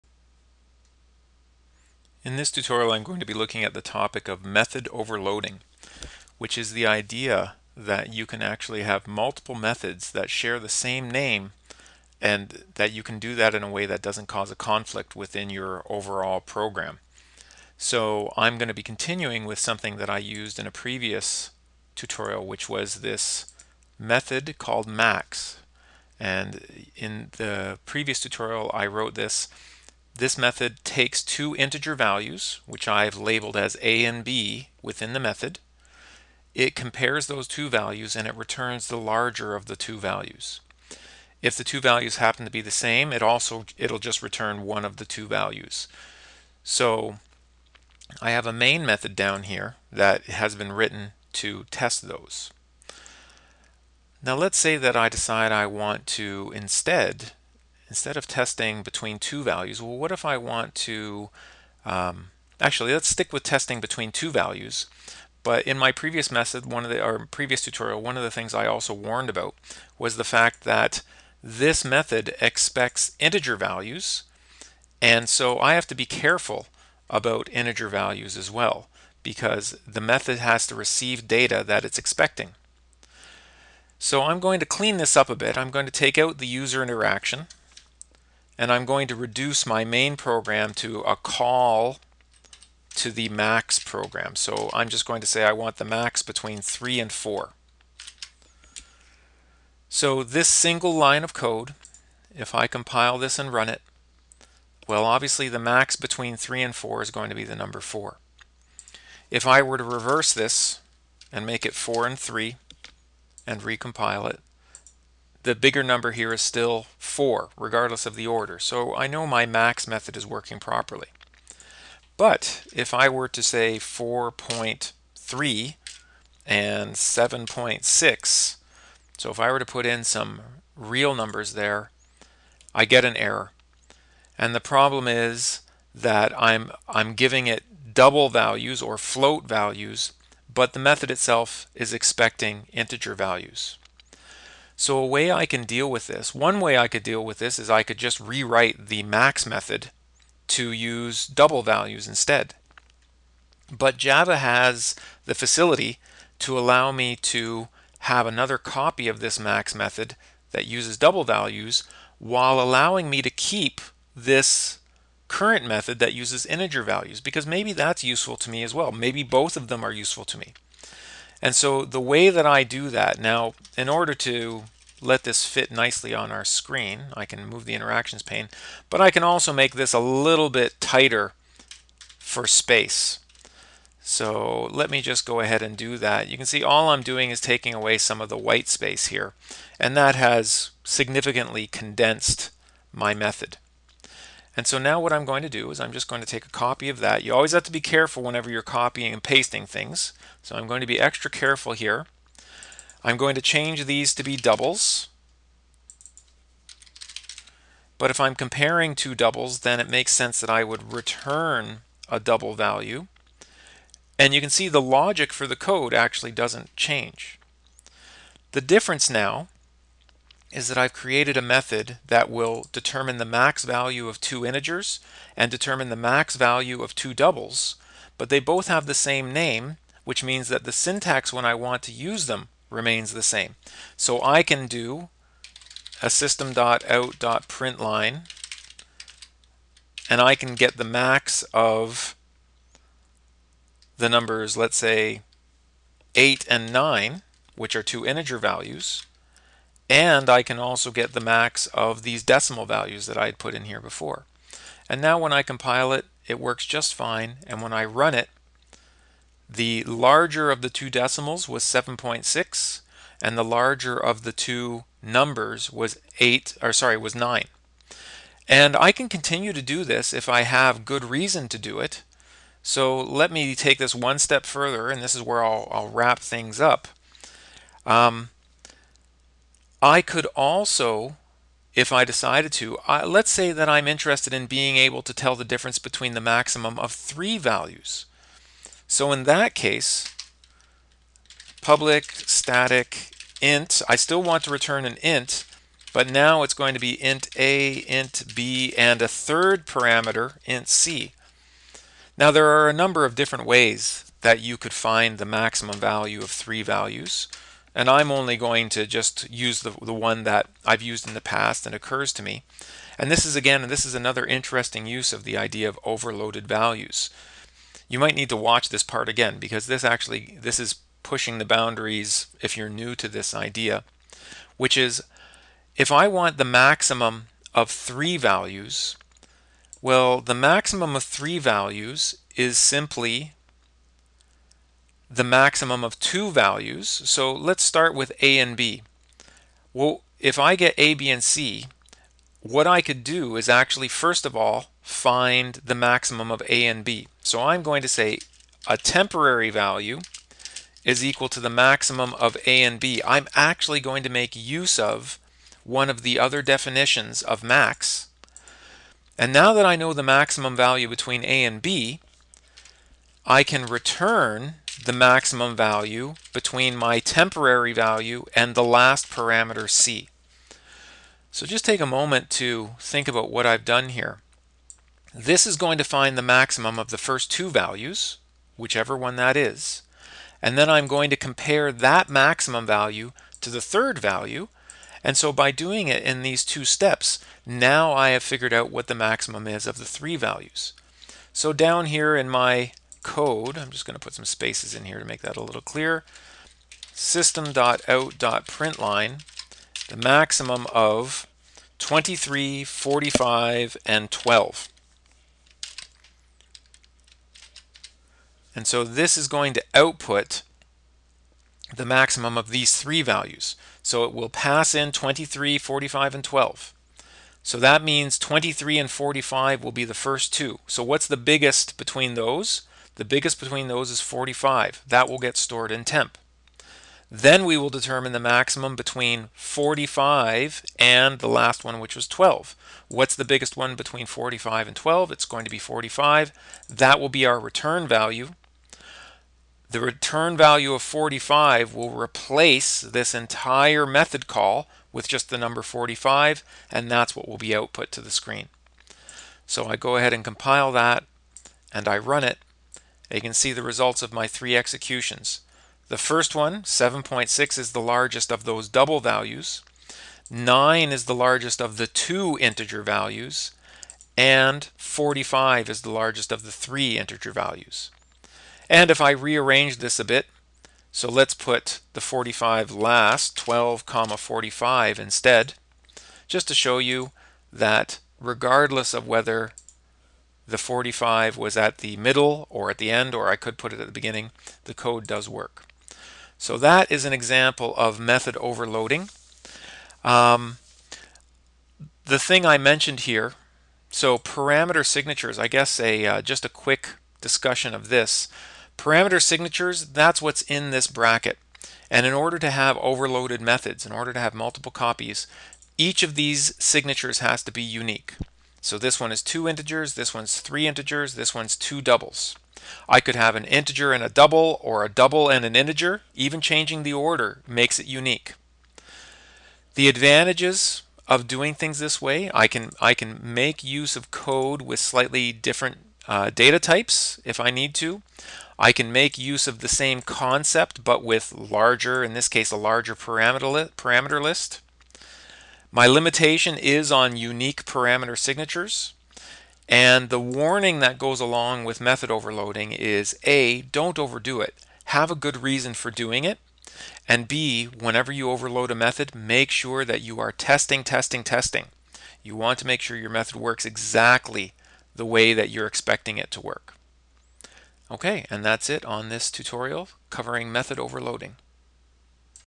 Java - Methods - Method Overloading
Java allows for multiple methods having the same name, or label.  These methods are distinguished from each other based on each having a unique parameter list.  This tutorial illustrates that concept by creating multiple versions of a "max" method.